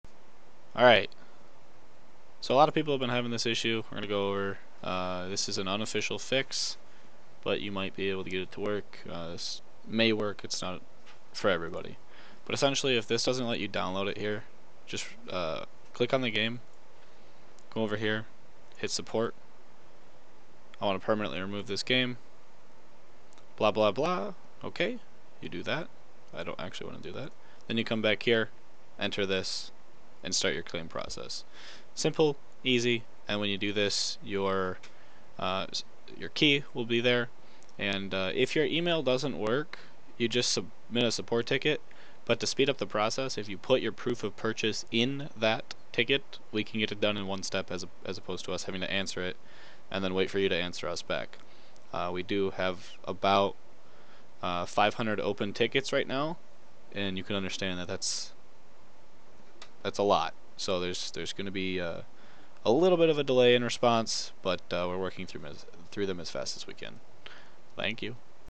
Unofficial fix for missing Steam Download Button for EmergeNYC
Hey everyone. User KickDown GTA and FlipSwitch Games Support team member Austin found a solution to having to wait for Steam to review our release. If you would like to play the game now, follow these steps. We do not guarantee it will work but it is worth a shot. Contact our support on our website if something goes wrong.